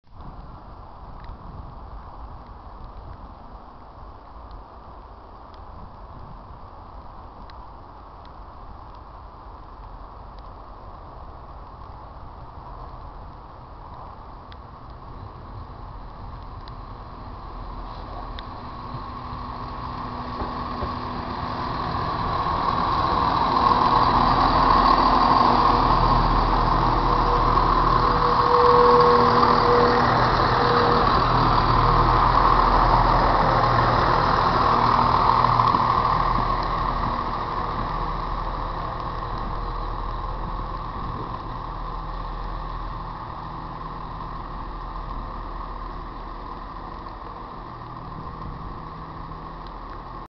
170106 1V12 07/11/13 Ashchurch for Tewkesbury
170106 is seen arriving at Ashchurch for Tewkesbury on the 07/11/13 with the 1V12 15:11 Nottingham-Cardiff Central XC service.